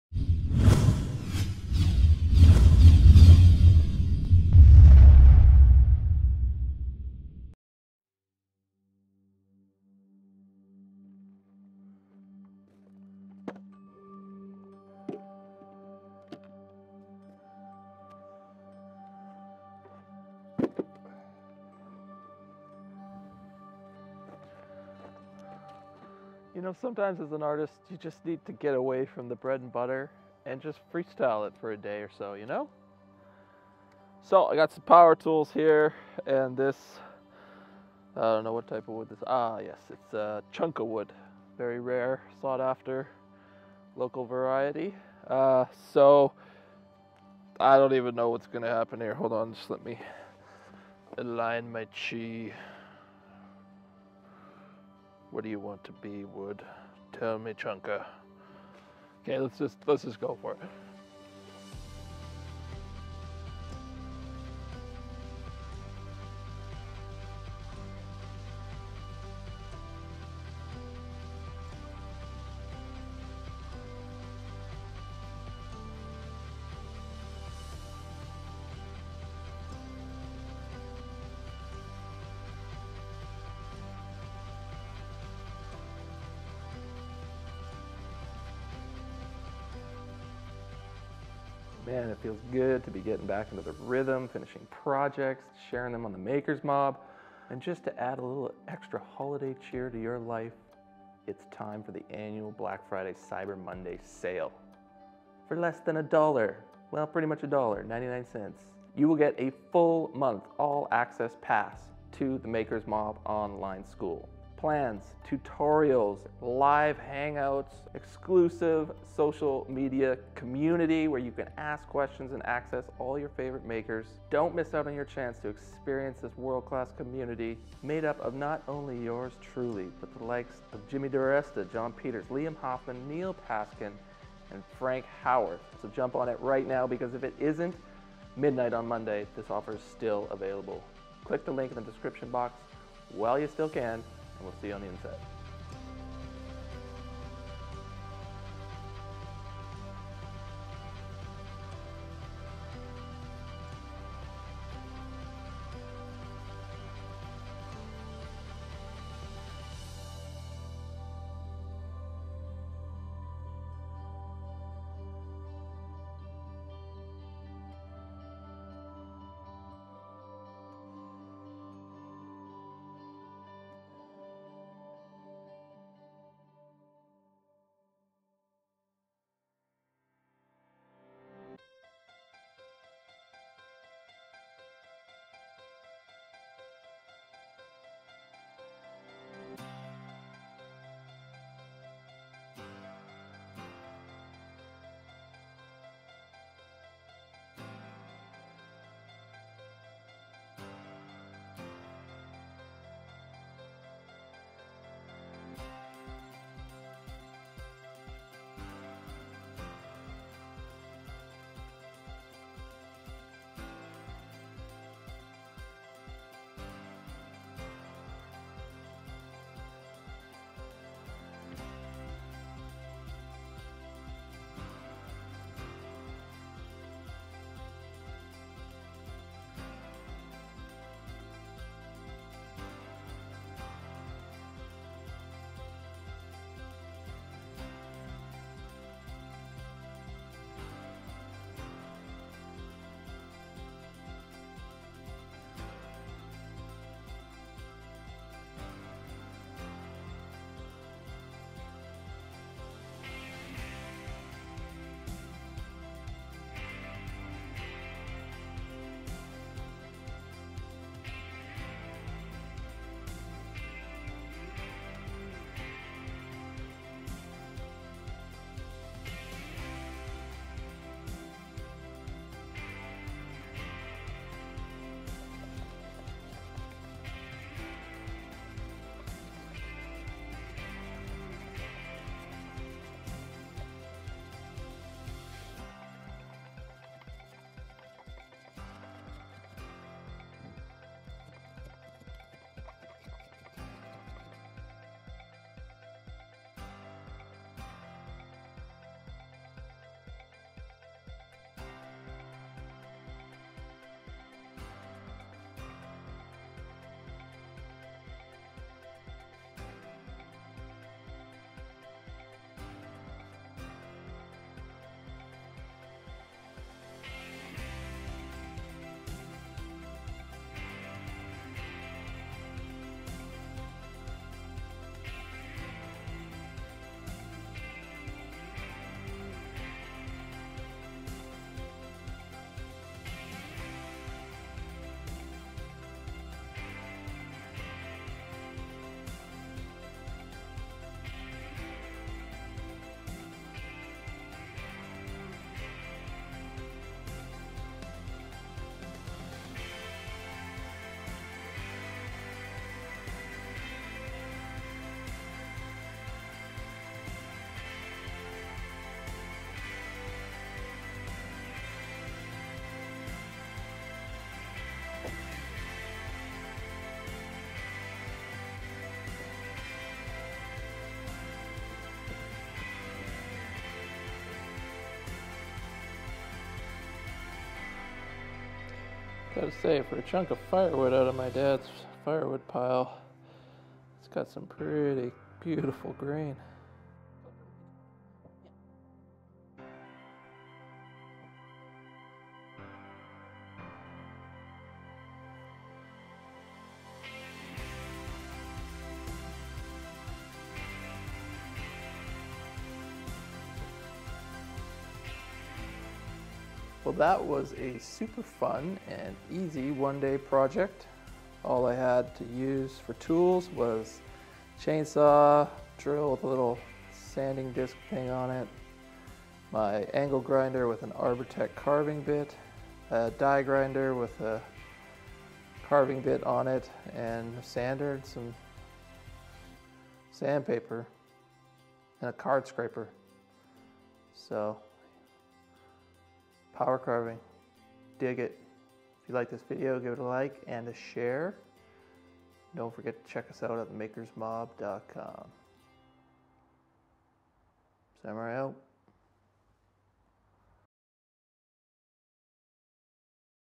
Carve a Wood Bowl - in 4 HOURS!
Sometimes you need to step away from the every day and just create… so I took four hours and set out to carve a wood bowl.  For this wood carving project I grabbed an old log from my dads wood pile and decided to work it and see what would happen.  Using my grinder and a few other tools I managed to carve something rough and rugged into what I consider a showpiece.  Watch along and see how I was able to carve a wood bowl in just four hours - Enjoy!